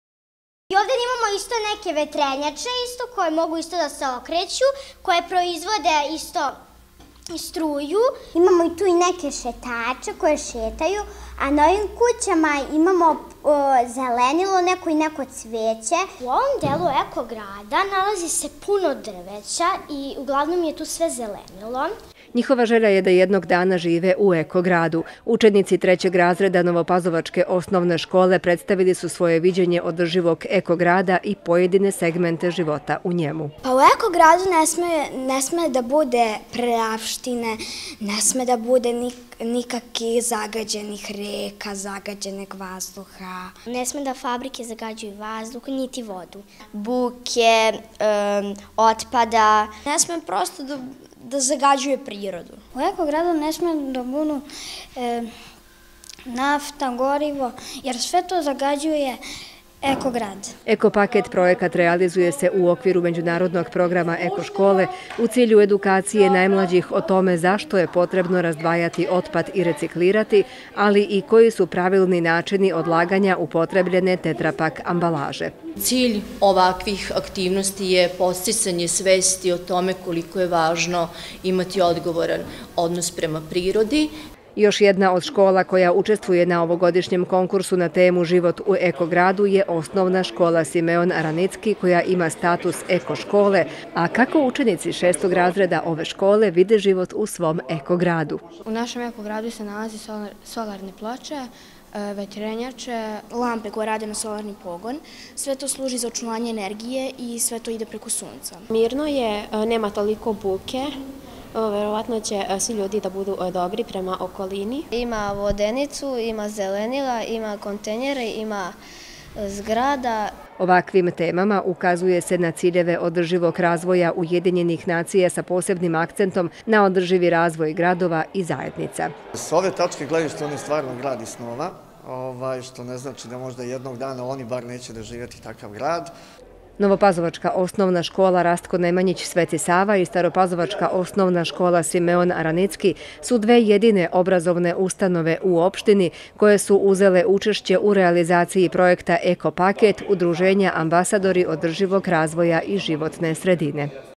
Kako izgleda život mladih u Eko gradu
Među 85 škola iz čitave Srvije, dve osnvne škole iz staropazovačke opštine učestvuju u realizaciji projekta „Eko Paket“ koji je ujedno deo Međunarodnog programa Eko Škole. Projekat je pokrenut prvo u Sloveniji, zatim u Hrvatskoj, a od školske 2013/2014. godine sprovodi se i u Srbiji s jasnim ciljem: da deca od najranijeg uzrasta nauče kako se pravilno odlaže Tetra Pak ambalaža kao jedan od načina očuvanja životne sredina i zdravlje.